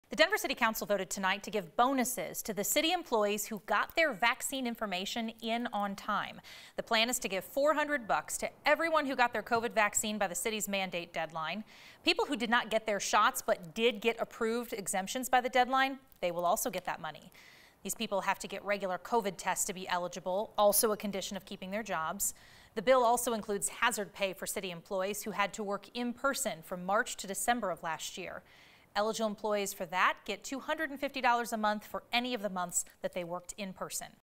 Denver city employees to receive $400 COVID vaccine bonuses, hazard pay
Denver City Council approved the proposals Monday. Eligible city employees will receive $400 COVID-19 vaccine bonuses and up to $2,500 in pandemic hazard pay.

9NEWS (KUSA) is located in Denver, Colorado.